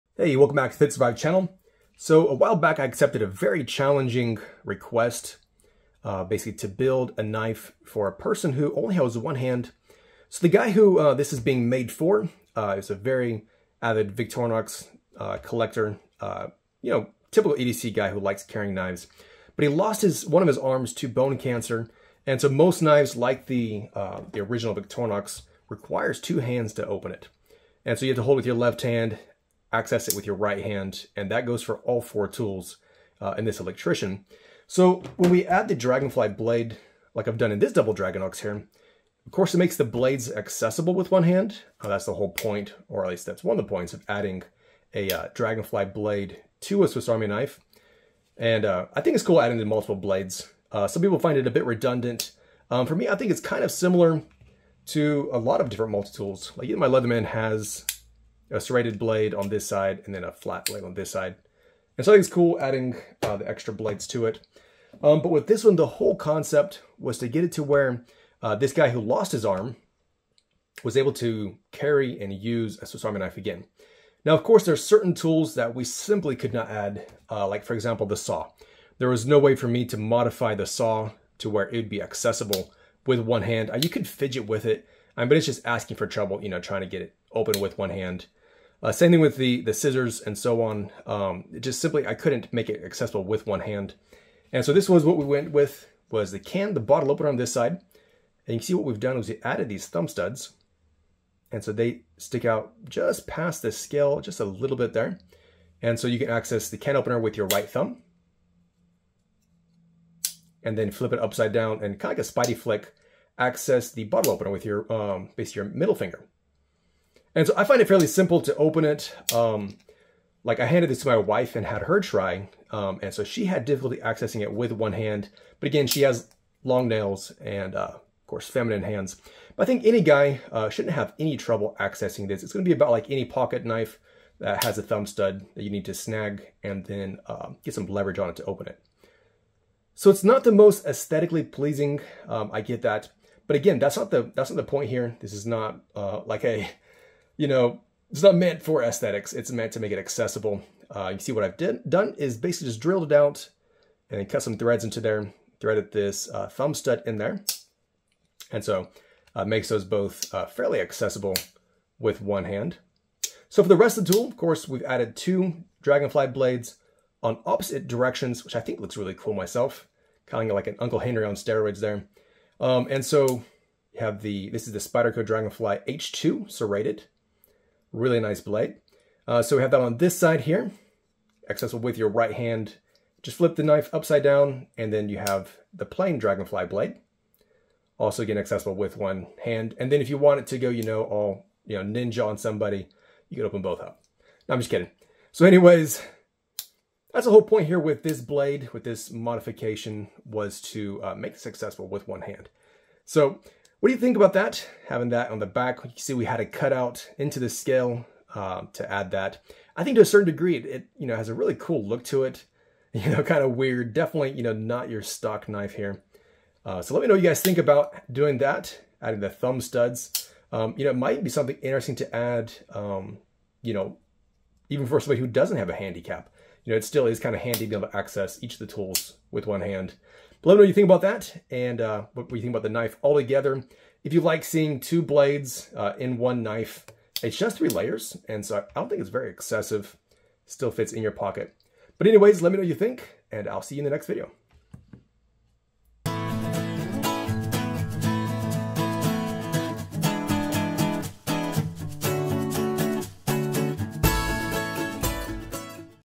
I made a custom SAK for an amputee | 4K
This custom build turned out really well, I was a bit concerned about the thumbstuds.  But what do you think?